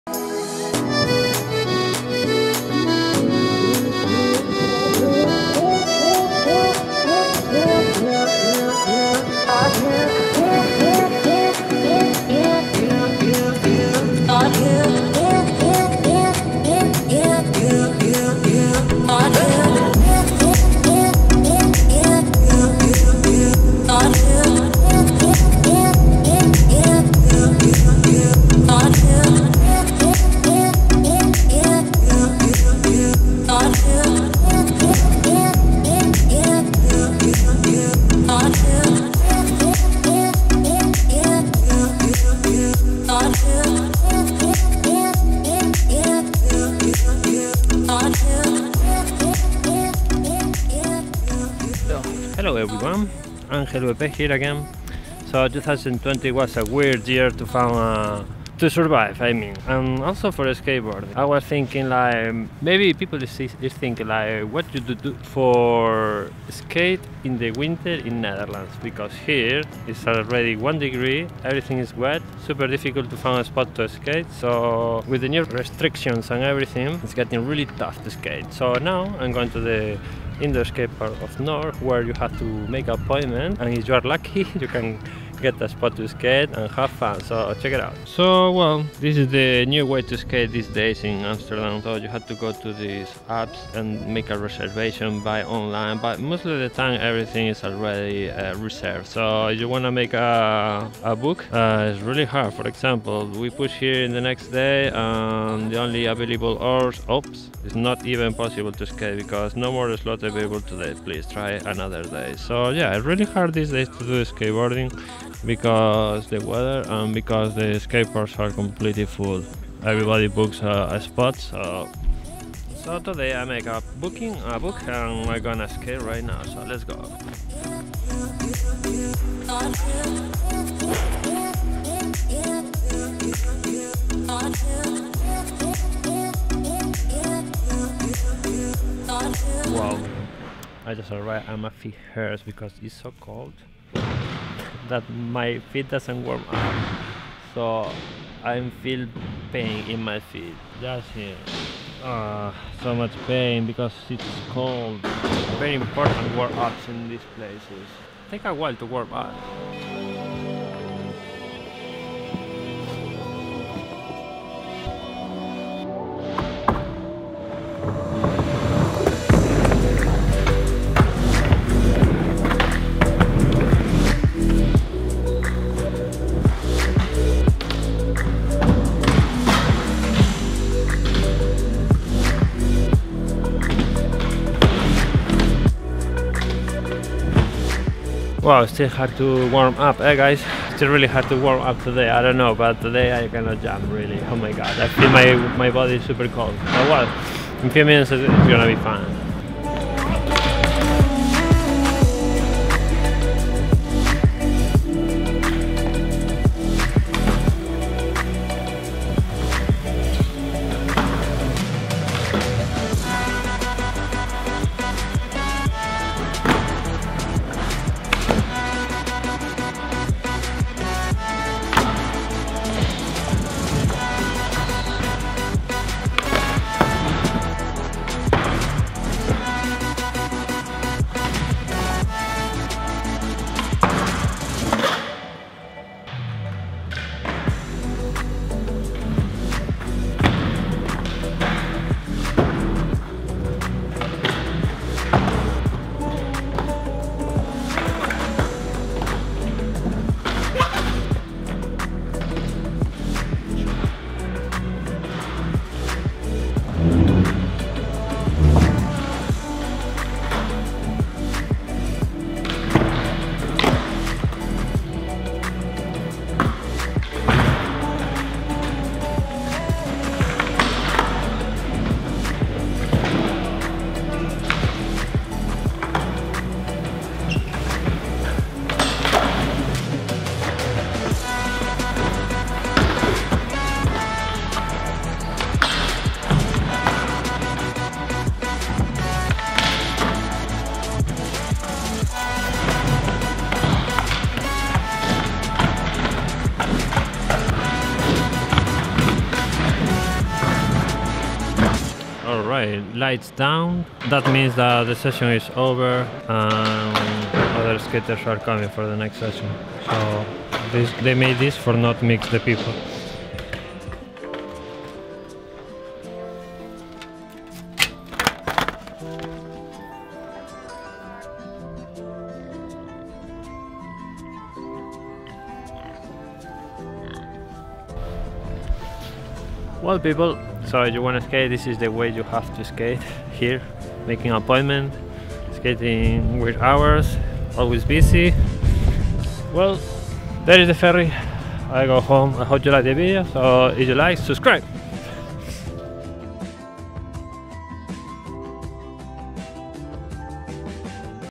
Skating (Noord indoor) in 2020 - Amsterdam ❄️
Join me for a trip back to 2020 as I skate at Noord Indoor in Amsterdam! ❄️
This year was nothing short of crazy, and one challenge we faced was the scarcity of skate spots during the harsh Dutch winters. In this video, I share some memorable anecdotes from that time and take you through the process we went through to keep skating before the second lockdown hit. It's a look at the resilience of our skate community and the joy of riding indoors.

Channel dedicated to everything that surrounds Angel V.P. recorded to share with people who want to be entertained for a good time. 😄😄😄